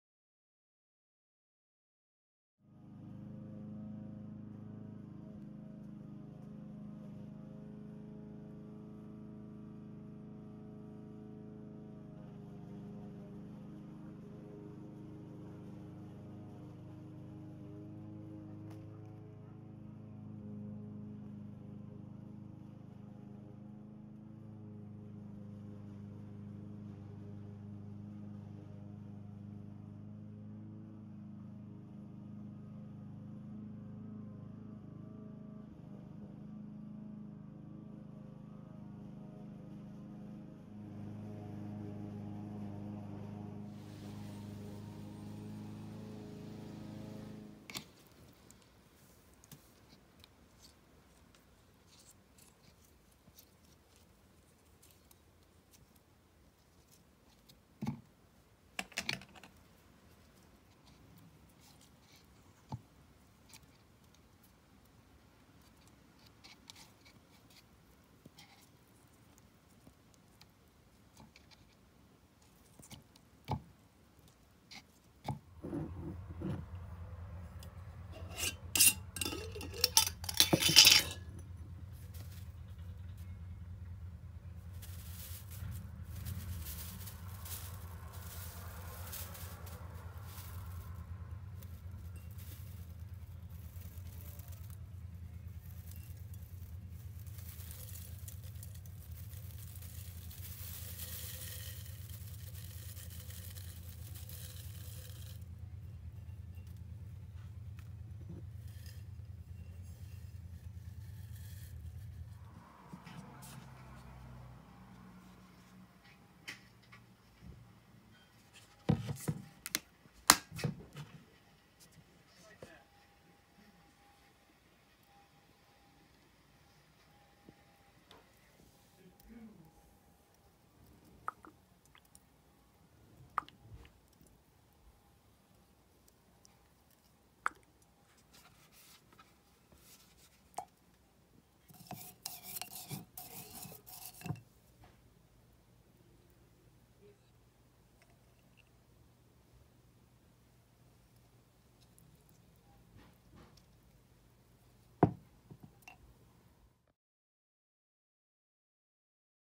How To Make Rose Infused Oil
1. Harvest rose petals.
2. Rinse petals and dry in mesh bag.
3. Put dried petals in jar.
4. Submerge dried petals in light oil.
5. Mix well.
6. Store in warm dark place for 3 weeks.